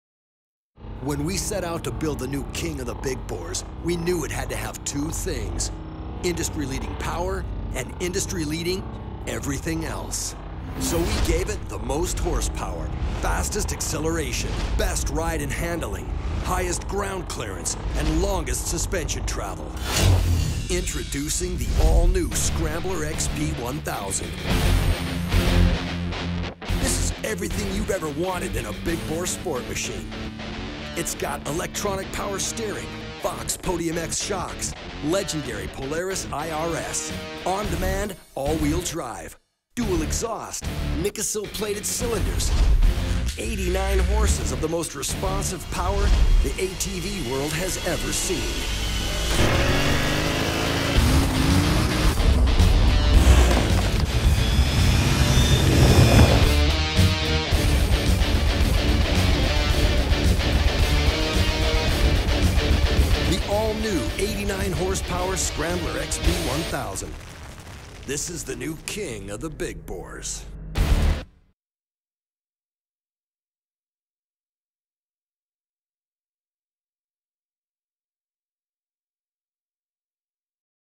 New 2014: Polaris Scrambler XP 1000
Introducing the 2014 Scrambler XP 1000. The Ultimate Sport Performance ATV.